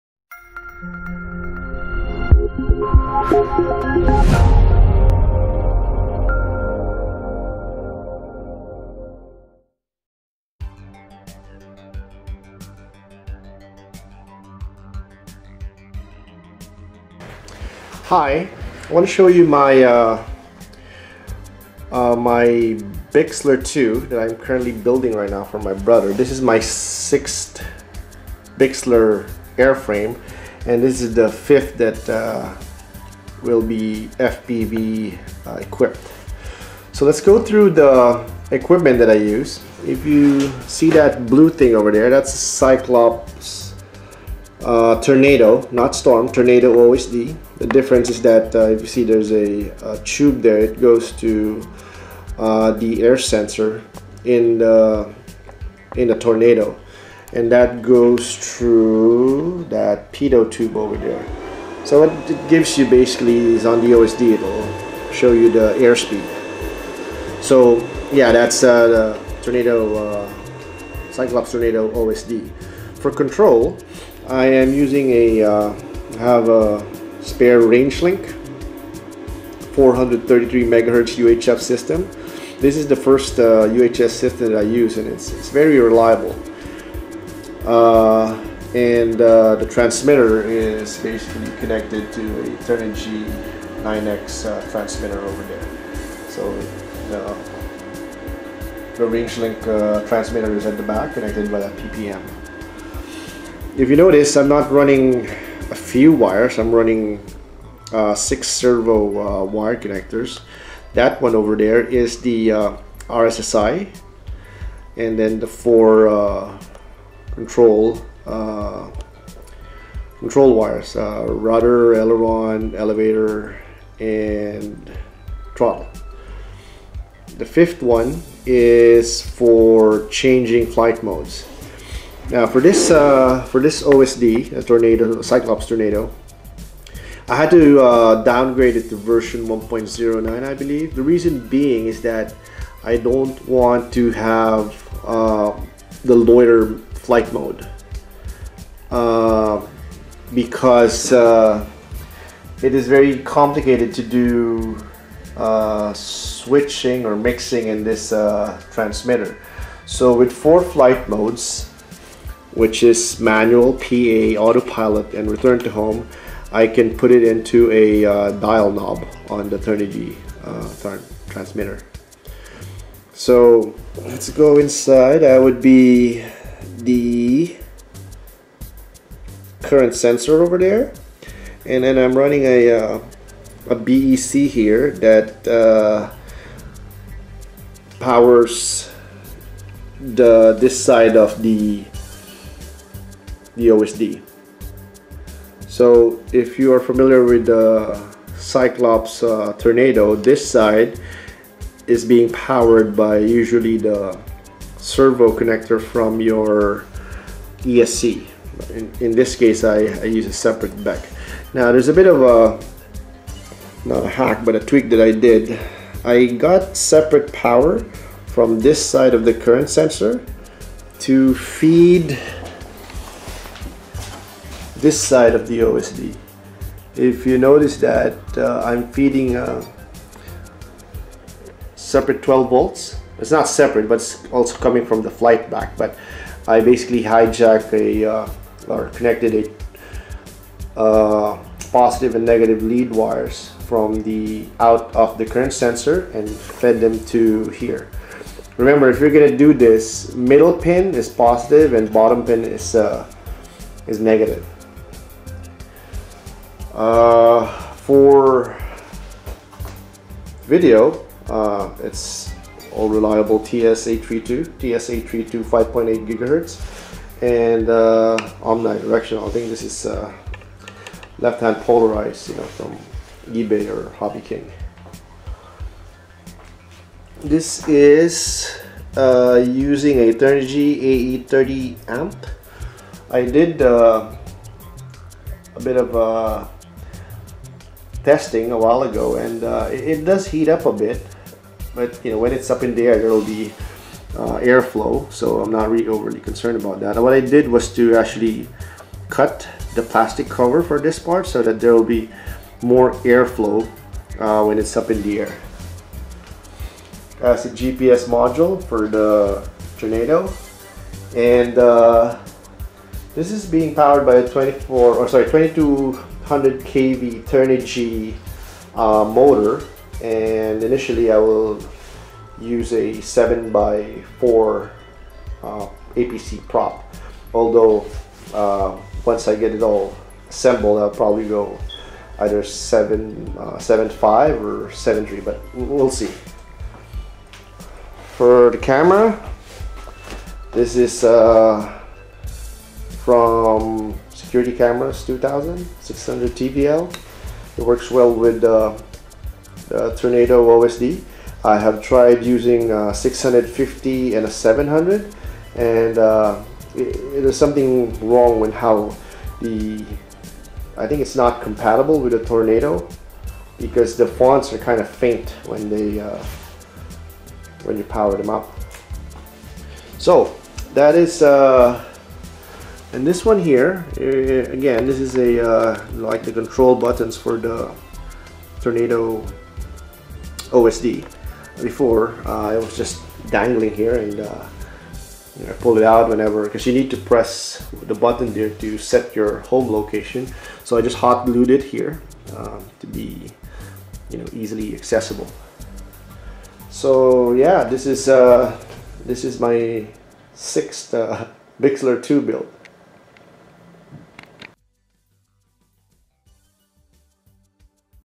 Bixler 2 + Cyclops Tornado Build on 5.8 GHz w/narration
Building my 6th Bixler 2 FPV airframe.
Prop: APC 7x5, 7x4
AP/OSD: Cyclops Tornado firmware v1.09
Servos: HXT900, Emax ES08MAII
ESC: Turnigy AE-30A
RC Control: Rangelink 12 channel 433MHz LRS
VTX: TS832 5.8 GHz
VRX: RC832 32 channels
FPV camera: Sony 600 TVL
HD Camera: Runcam HD